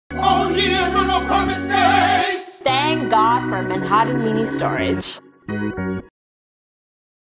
Oh Ye of Little Apartment Space - MiniStories - Manhattan Mini Storage
When you have no place to store your inventory put your faith in storage.